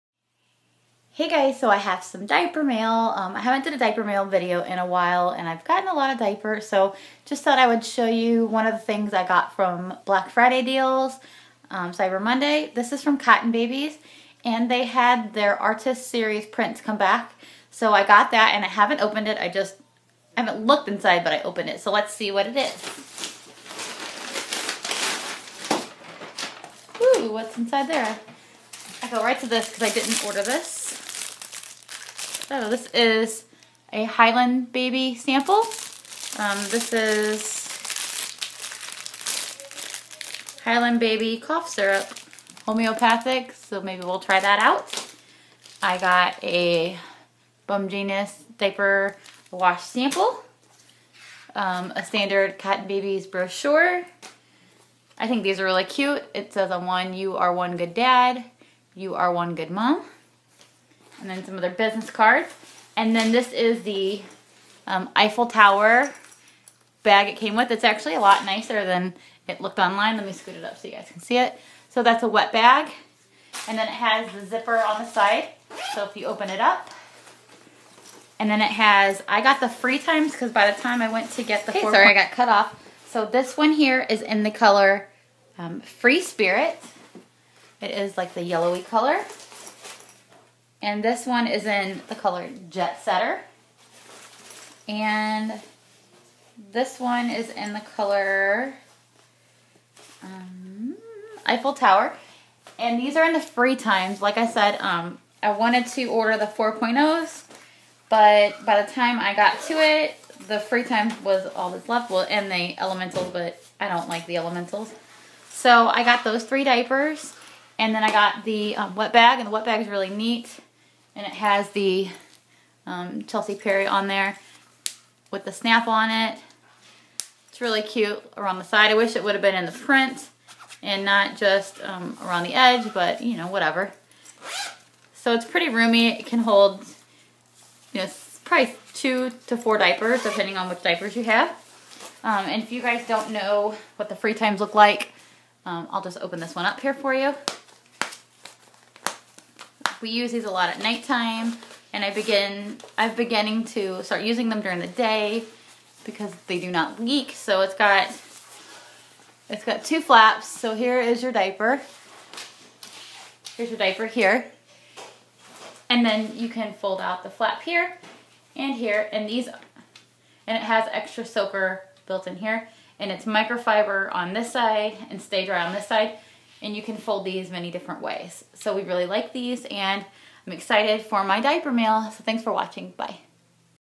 Cotton Babies Diaper Mail
Some diaper mail from Black friday/Cyber monday deals.